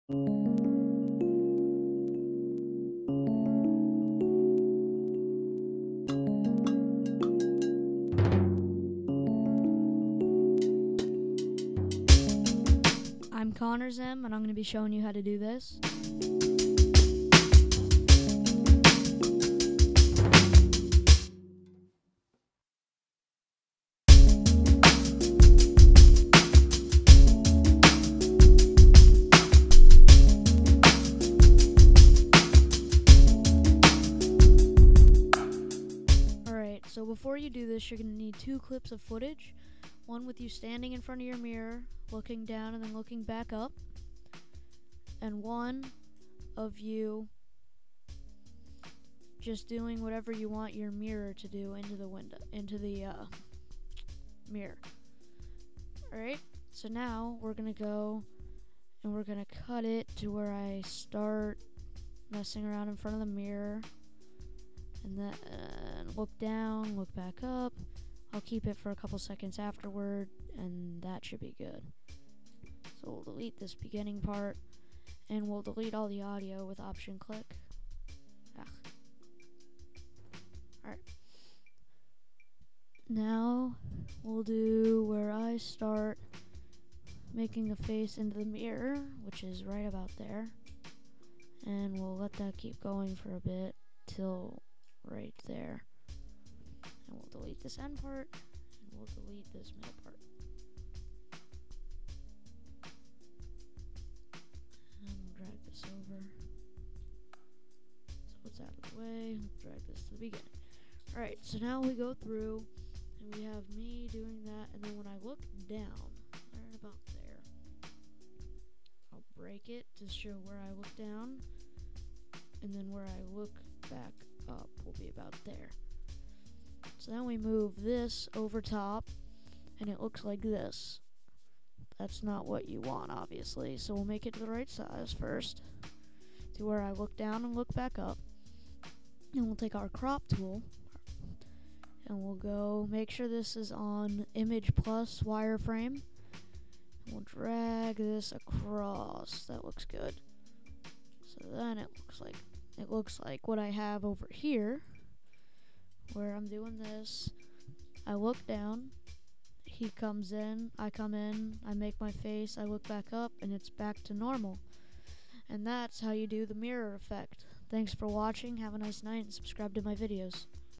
Final Cut Express Tutorial: Mirror Effect
Here's how to do an effect that I call the mirror effect.  It is a really cool effect which is used in Mirrors.  Enjoy!!